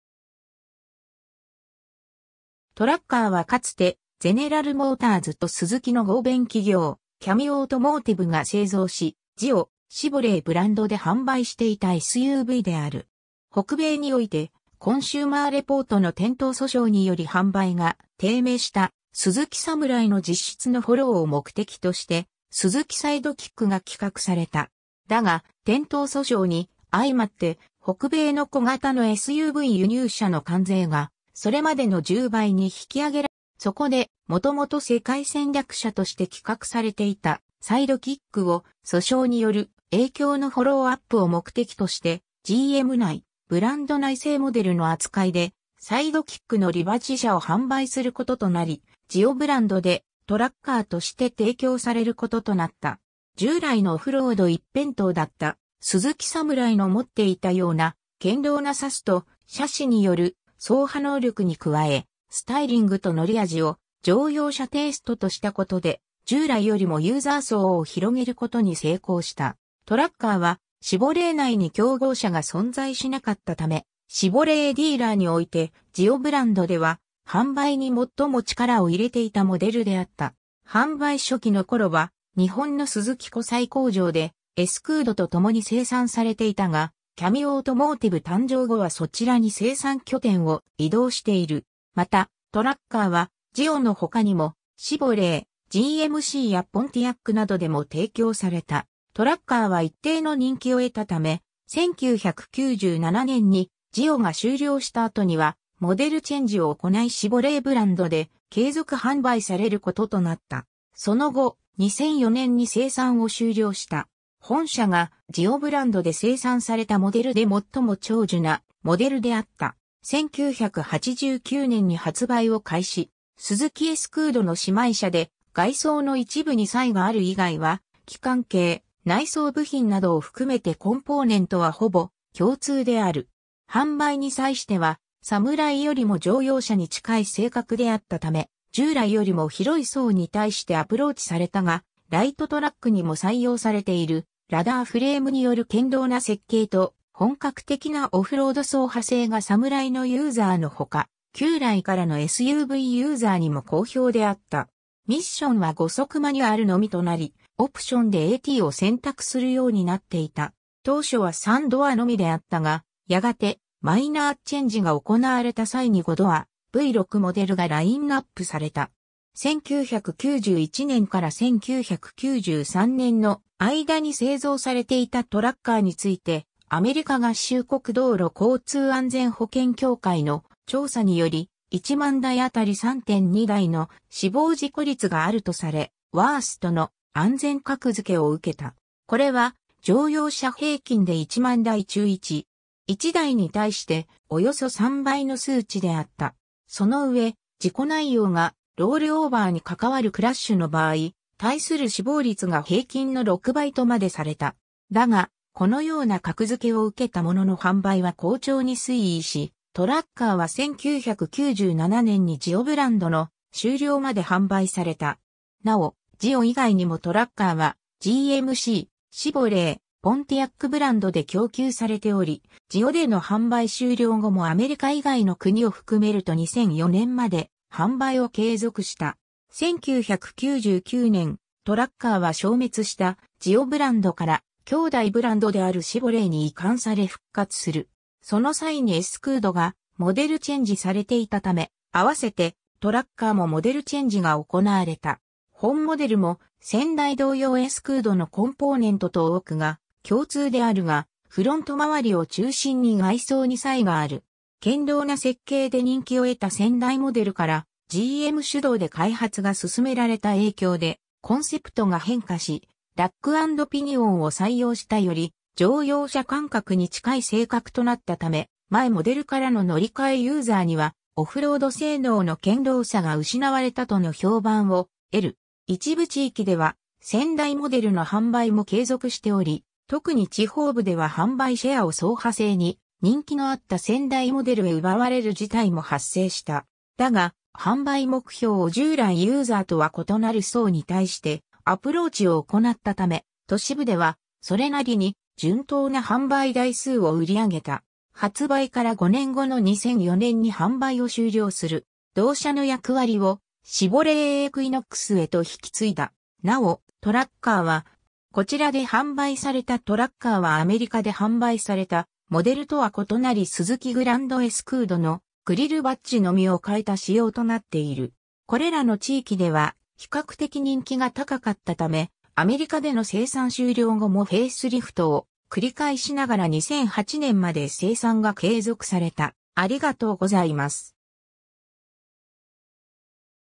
ジオ・トラッカー
トラッカー（Tracker ）はかつてゼネラルモーターズとスズキの合弁企業「CAMIオートモーティブ」が製造し、「ジオ」、「シボレー」ブランドで販売していたSUVである。
 北米において、コンシューマー・レポートの転倒訴訟により販売が低迷した「スズキ・サムライ」の実質のフォローを目的として「スズキ・サイドキック（スズキ・エスクード）」が企画された。
だが、転倒訴訟にあいまって北米の小型のSUV輸入車の関税がそれまでの10倍に引き上げられたこともあり、スズキブランドそのままの状態での販売が難しくなっていた。
そこでもともと世界戦略車として企画されていたサイドキックを、訴訟による影響のフォローアップを目的として、GM内ブランド内製モデルの扱いでサイドキックのリバッジ車を販売することとなり、ジオブランドで「トラッカー」として提供されることとなった。
 従来のオフロード一辺倒だった「スズキ・サムライ」の持っていたような堅牢なサスとシャシによる走破能力に加え、スタイリングと乗り味を乗用車テイストとしたことで、従来よりもユーザー層を広げることに成功した。
 トラッカーはシボレー内に競合車が存在しなかったため、シボレーディーラーにおいてジオブランドでは販売に最も力を入れていたモデルであった。
 販売初期のころは日本のスズキ湖西工場でエスクードとともに生産されていたが、CAMIオートモーティブ誕生後はそちらに生産拠点を移動している。
 また、トラッカーはジオの他にもシボレー、GMCやポンティアックなどでも提供された。
 トラッカーは一定の人気を得たため、1997年にジオが終了した後には、モデルチェンジを行いシボレーブランドで継続販売されることとなった。
その後、2004年に生産を終了した。
 本車がジオブランドで生産されたモデルで最も長寿なモデルであった。
 1989年に発売を開始。
スズキ・エスクードの姉妹車で、外装の一部に差異がある以外は機関系、内装部品などを含めてコンポーネントはほぼ共通である。
 販売に際してはサムライよりも乗用車に近い性格であったため従来よりも広い層に対してアプローチされたが、ライトトラックにも採用されているラダーフレームによる堅牢な設計と本格的なオフロード走破性がサムライのユーザーのほか、旧来からのSUVユーザーにも好評であった。
 ミッションは5速マニュアルのみとなり、オプションでATを選択するようになっていた。
当初は3ドアのみであったが、やがてマイナーチェンジが行われた際に5ドア、V6モデルがラインナップされた。
 1991年から1993年の間に製造されていたトラッカーについて、アメリカ合衆国道路交通安全保険協会の調査により、1万台あたり3.2台の死亡事故率があるとされ「ワースト」の安全格付けを受けた。
 これは乗用車平均で1万台中1.1台に対しておよそ3倍の数値であった。
 その上、事故内容がロールオーバーに関わるクラッシュの場合、対する死亡率が平均の6倍とまでされた。
だがこのような格付けを受けたものの販売は好調に推移し、トラッカーは1997年にジオブランドの終了まで販売された。
 なお、ジオ以外にもトラッカーはGMC、シボレー、ポンティアックブランドで供給されており、ジオでの販売終了後もアメリカ以外の国を含めると2004年まで販売を継続した。
 1999年、トラッカーは消滅したジオブランドから兄弟ブランドであるシボレーに移管され復活する。
その際にエスクードがモデルチェンジされていたため、併せてトラッカーもモデルチェンジが行われた。
 本モデルも先代同様エスクードのコンポーネントと多くが共通であるが、フロント周りを中心に外装に差異がある。
 堅牢な設計で人気を得た先代モデルから、GM主導で開発が進められた影響でコンセプトが変化し、ラックアンドピニオンを採用したより乗用車感覚に近い性格となったため、前モデルからの乗り換えユーザーにはオフロード性能の堅牢さが失われたとの評判を得る。
 一部地域では先代モデルの販売も継続しており、特に地方部では販売シェアを走破性に人気のあった先代モデルへ奪われる事態も発生した。
だが販売目標を従来ユーザーとは異なる層に対してアプローチを行ったため、都市部ではそれなりに順当な販売台数を売り上げた。
 発売から5年後の2004年に販売を終了する。
同車の役割をシボレー・エクイノックスへと引き継いだ。
 なお、トラッカーは...